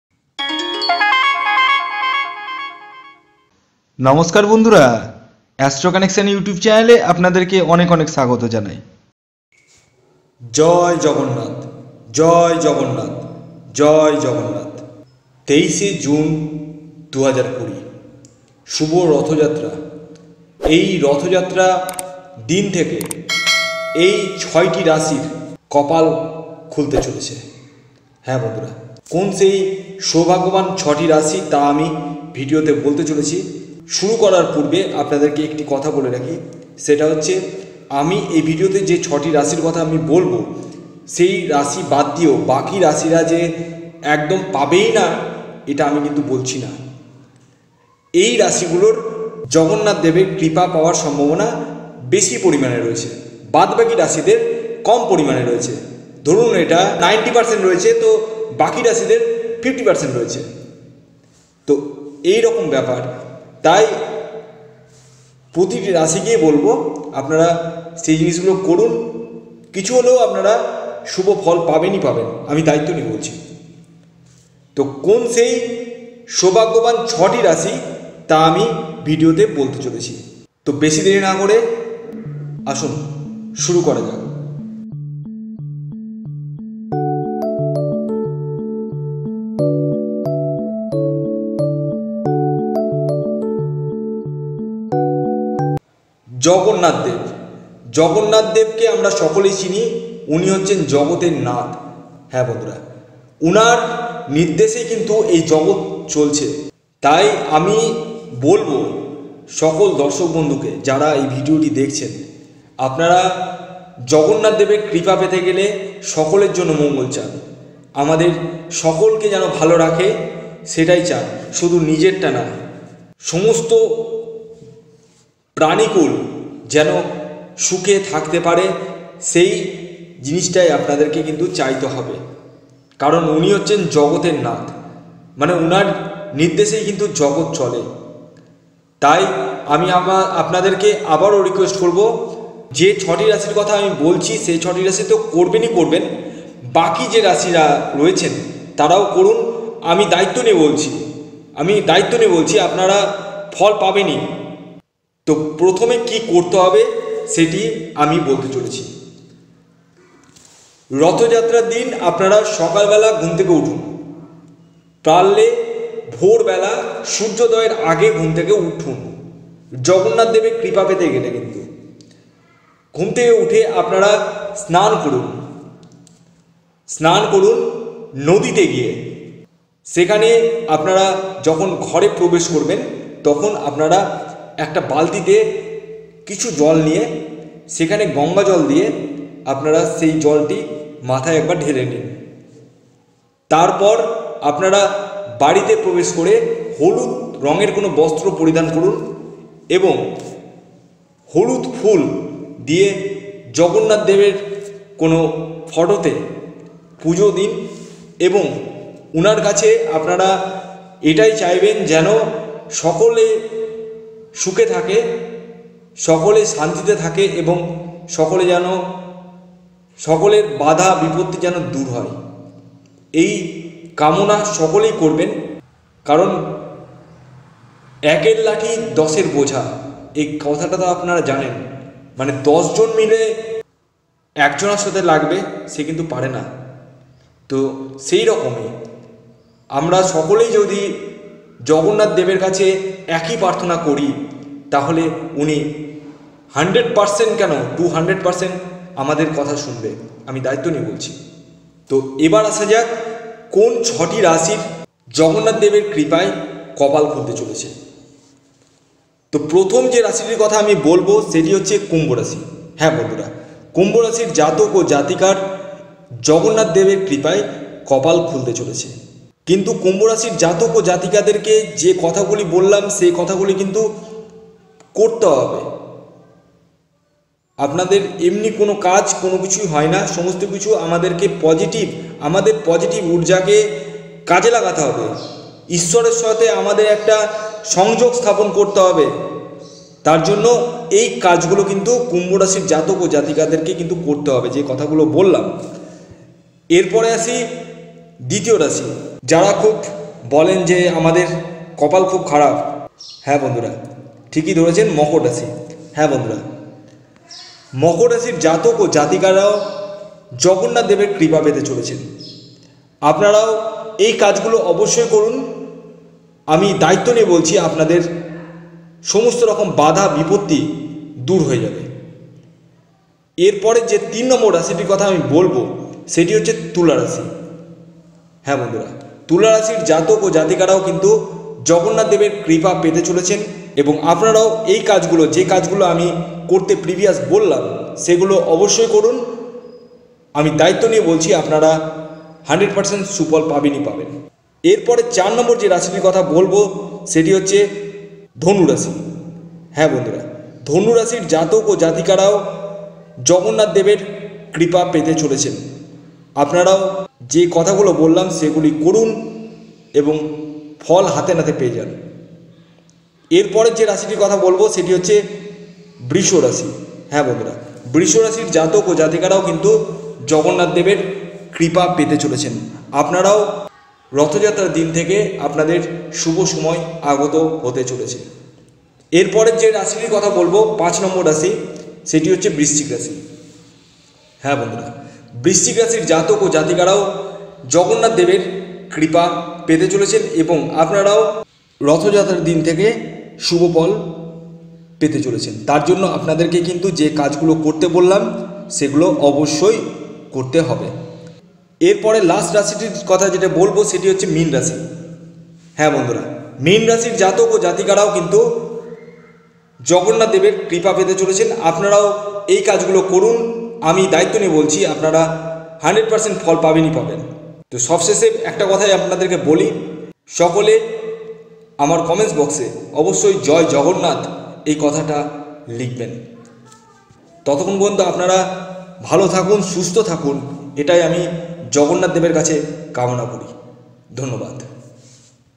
শুভ রথযাত্রা থেকে এই 6টি রাশি স্বয়ং জগন্নাথ দেবের কৃপা পেতে চলেছে।।
বন্ধুরা এই ভিডিওতে আমি বলেছি 23June 2020 শুভ রথযাত্রা এই দিন থেকে এই 6টি রাশি স্বয়ং জগন্নাথ দেবের কৃপা পেতে চলেছে।
কোন সেই ভাগ্যবান 6টি রাশি জানতে ভিডিওটি পুরো দেখুন।